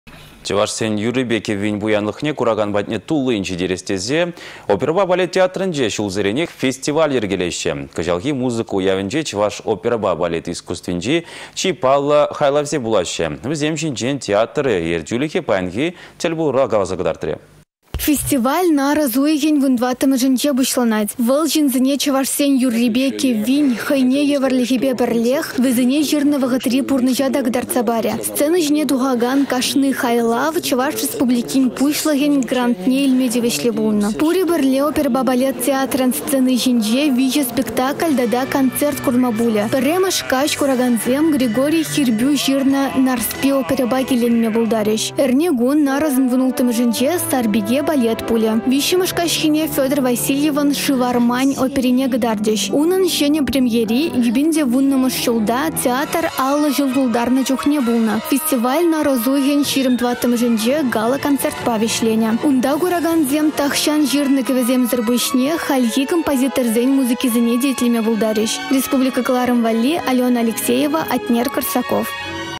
Чăваш юррипе кĕввин кунĕсем
Чăвашсен юррипе кĕввин пуянлăхне куракан патне туллин çитерес тесе оперăпа балет театрĕнче çулсеренех фестиваль йĕркелеççĕ. Кăçалхи музыка уявĕнче чăваш оперăпа балет искусствинчи чи паллă хайлавсем пулаççĕ. Вĕсем çинчен театр ертÿлĕхĕ паянхи тĕл пулура каласа кăтартрĕ.